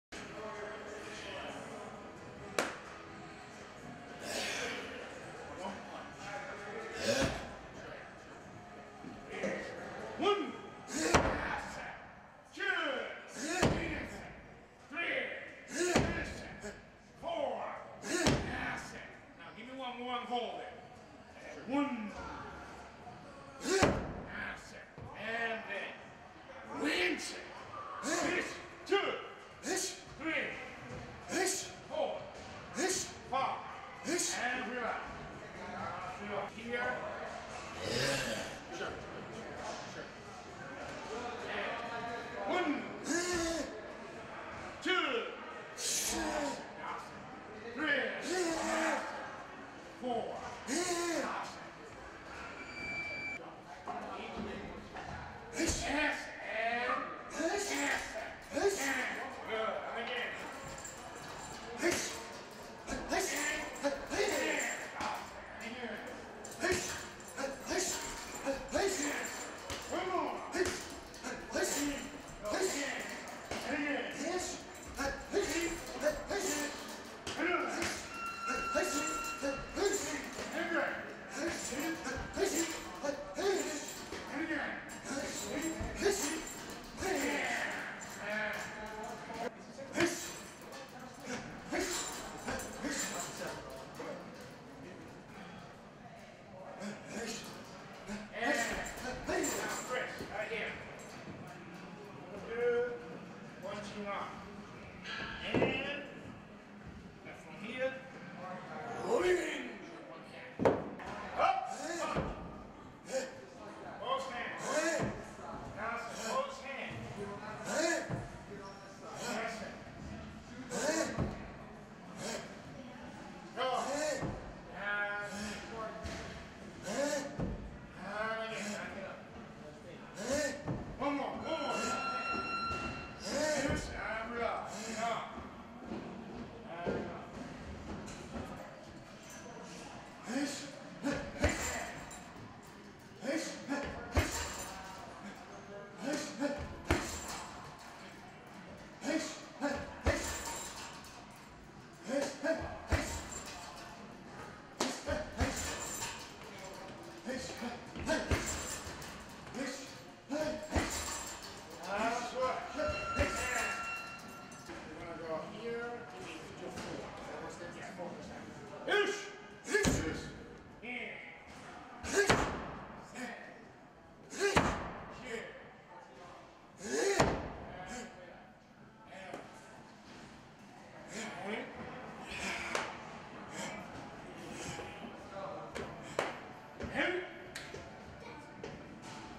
Chris Dellarocco an MMA Competitor
Chris Dellarocco from Calasanz Physical Arts demonstates his MMA training knee strikes, ground exercises, and round kicks